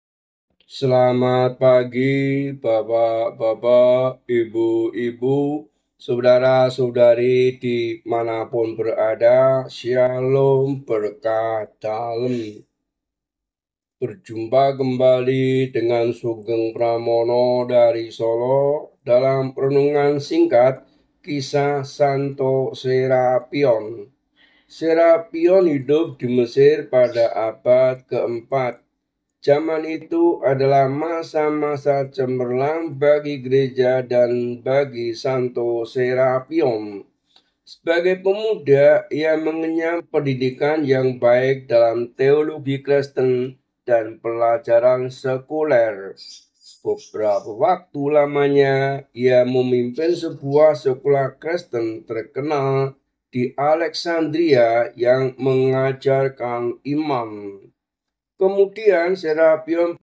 Short Santo Serapion | 21 Maret | Sugeng Pramono
SANTO  SERAPION

Serapion of Thmuis, Serapion the Scholar

Serapion hidup di Mesir pada abad keempat. Jaman itu adalah masa-masa cemerlang bagi Gereja dan bagi Santo Serapion.

Sebagai pemuda, ia mengenyam pendidikan yang baik dalam teologi Kristen dan pelajaran sekuler. Beberapa waktu lamanya ia memimpin sebuah sekolah Kristen terkenal di Alexandria yang mengajarkan iman.

Kemudian Serapion pergi ke padang gurun dan menjadi seorang pertapa. Ia bertemu dengan seorang pertapa terkenal, Santo. Antonius dari Mesir. Serapion berjuang keras untuk belajar darinya dan meneladaninya. Ketika meninggal, Santo.Antonius mewariskan bagi Serapion salah satu jubahnya, yang disimpan baik oleh Serapion sepanjang hidupnya.

Serapion ditahbiskan sebagai Uskup Thmuis, sebuah kota di Mesir. Ia ikut ambil bagian dalam suatu pertemuan penting para uskup di Sardica pada tahun 347. Serapion membuktikan diri sebagai seorang uskup yang gagah berani.

Ia mencintai kebenaran-kebenaran iman dan berusaha melindunginya dari orang-orang yang hendak mengubah keyakinan Kristen. Santo. Serapion bekerjasama dengan Santo Atanasius, seorang uskup gagah lainnya. Kedua orang kudus ini terkenal karena keberanian mereka.

Mereka berperang melawan ajaran-ajaran sesat ( Arian ) atau bidaah-bidaah dengan homili-homili dan tulisan-tulisan mereka. Sebagian besar tulisan Santo. Serapion kini telah hilang.

Karya tulisnya berupa surat-surat penuh pengajaran iman dan penjelasan Mazmur. Karyanya yang terpenting, “Euchologion”, yang telah hilang selama beratus-ratus tahun, ditemukan kembali dan kemudian dipublikasikan di akhir abad kesembilan belas.

Seorang santo terkenal lainnya pada masa itu, Santo. Hieronimus, mengatakan bahwa Kaisar Konstantius mengirim Serapion ke pembuangan. Tampaknya Serapion wafat sekitar tahun 370 di tempat pengasingan.

Perayaan    21 Maret

Salam Peradaban Kasih : Sugeng Pramono